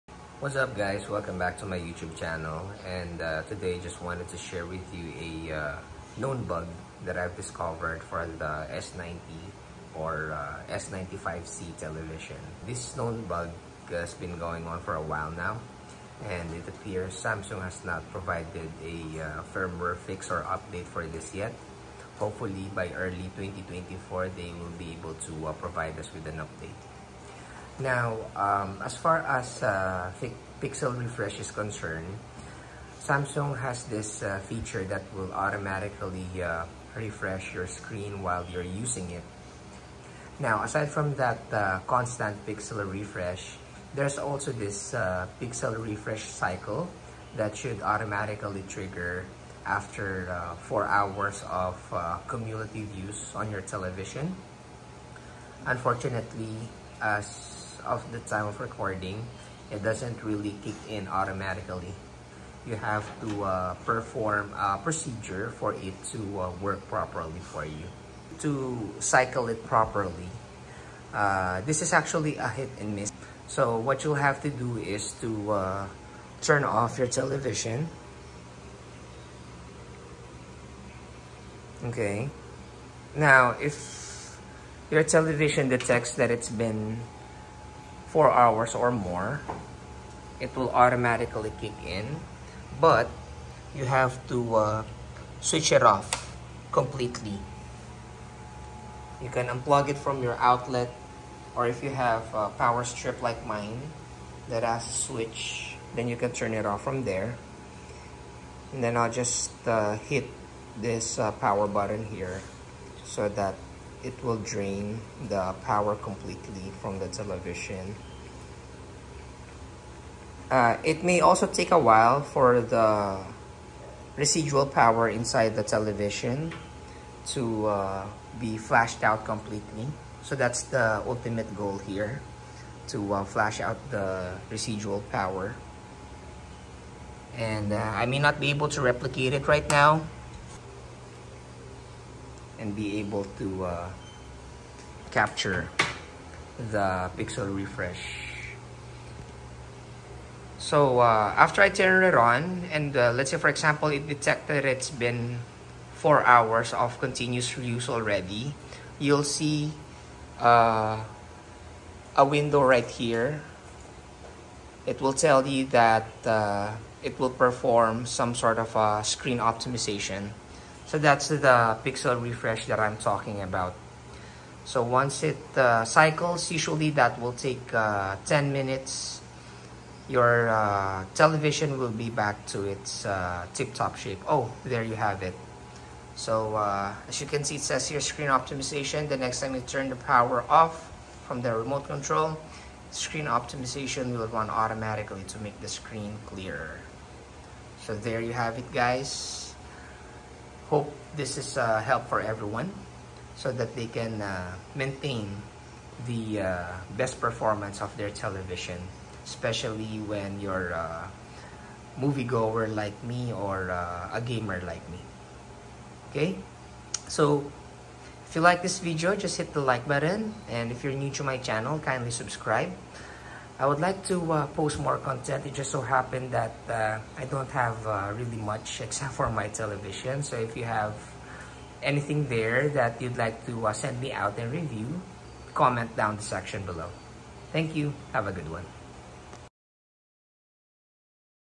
Oled Burn-in Risk! Samsung S90c/S95c Pixel Refresh Bug/Defect for QD-Oled Unfolds!
Defect or bug? Burn-in risk as pixel refresh cycle fails